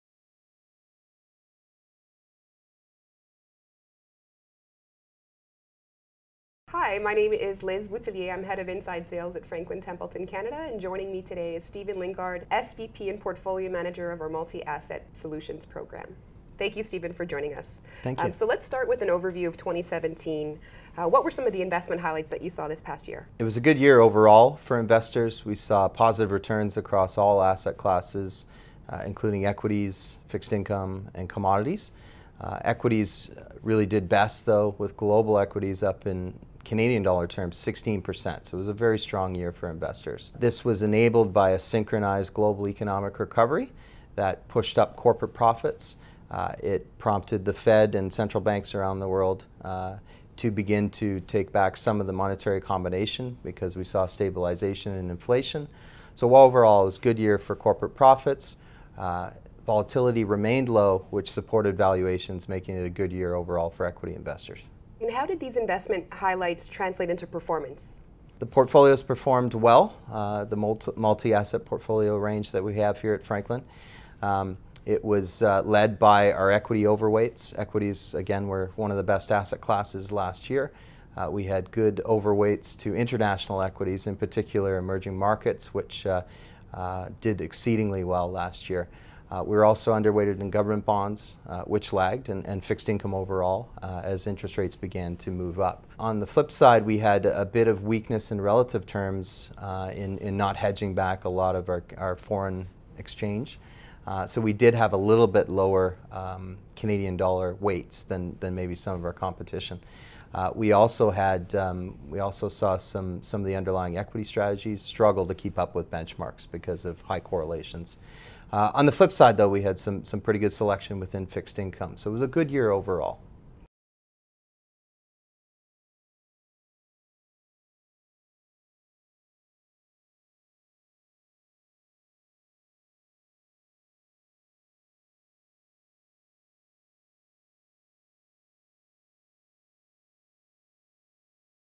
State of the Markets Q1 2018 - 2017 in Review
In this State of the Markets video, Franklin Quotential’s Stephen Lingard reviews how global markets performed in 2017, some of the macroeconomic trends that contributed to the strong performance of global markets, and how that translated into fund performance.
Recorded in January 2018 All investments involve risks, including possible loss of principal.